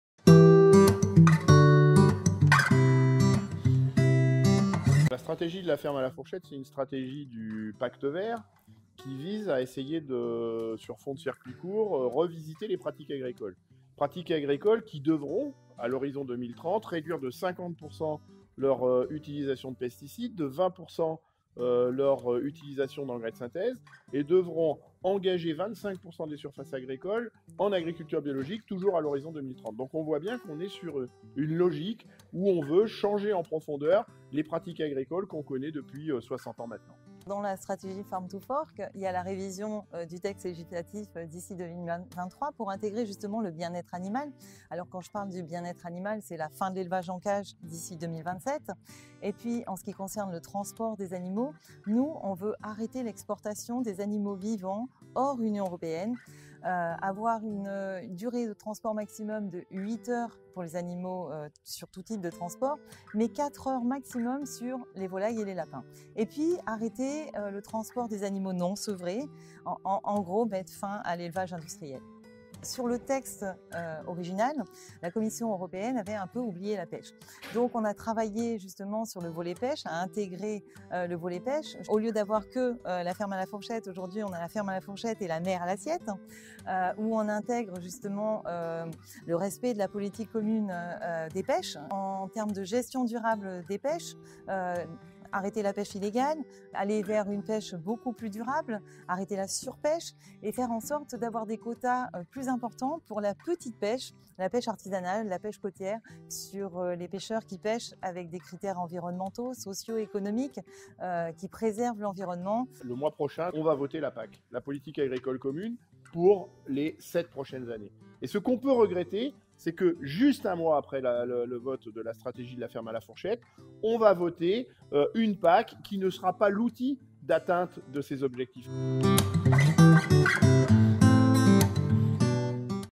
Caroline Roose et Benoît Biteau sur la Stratégie de la ferme à la fourchette
Cet après-midi, le Parlement européen a débattu de la stratégie de la Ferme à la fourchette. Les explications de Caroline Roose et Benoît Biteau vous en expliquent les enjeux.